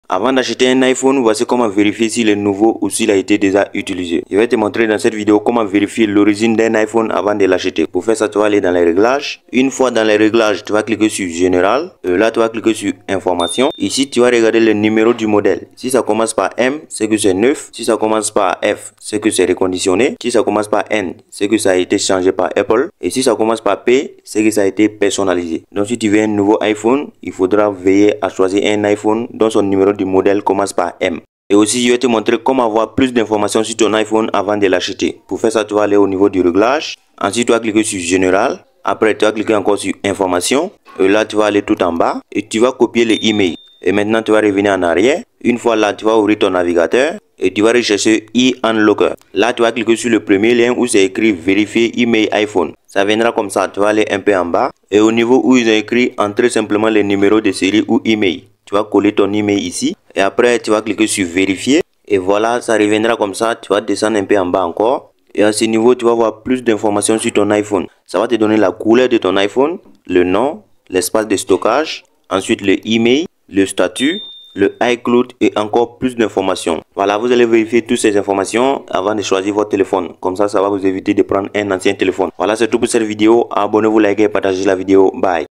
COMMENT VERIFIER L'ORIGINE D'UN IPHONE AVANT DE L'ACHETER
Site : www/huberica/com
tuto, tutorial, tutoriels, astuce, astuces, android, iphone, application